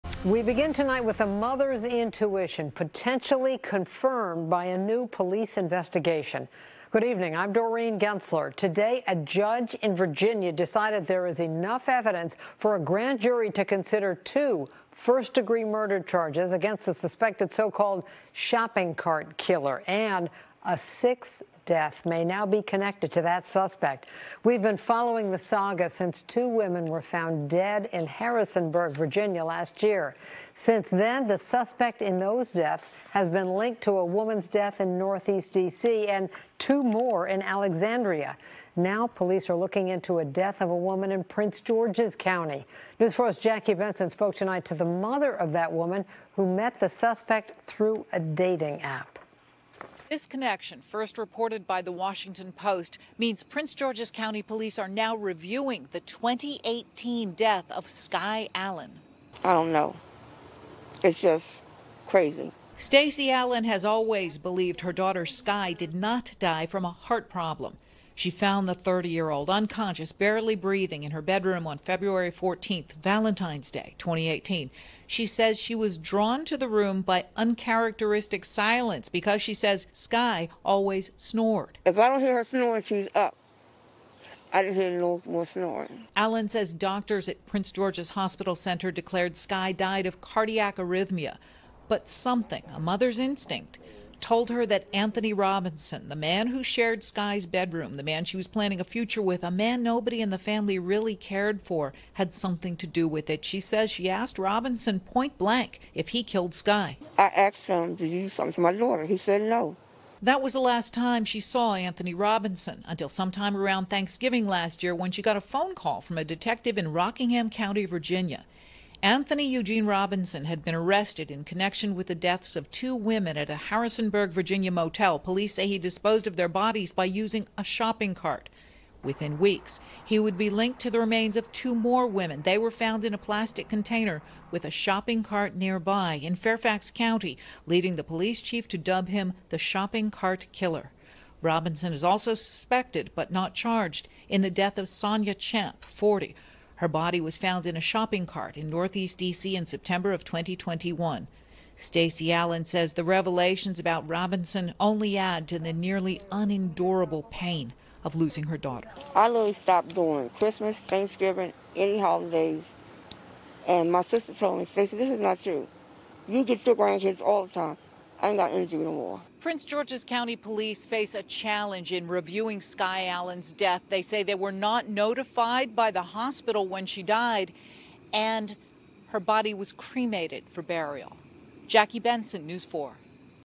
‘Shopping Cart Killer' Could Be Connected to Sixth Woman's Death | NBC4 Washington
A judge in Virginia decided that there is enough evidence for a grand jury to consider two first-degree murder charges against the suspected “shopping cart killer,” and a sixth death may now be connected to the suspect. News4’s Jackie Bensen reports.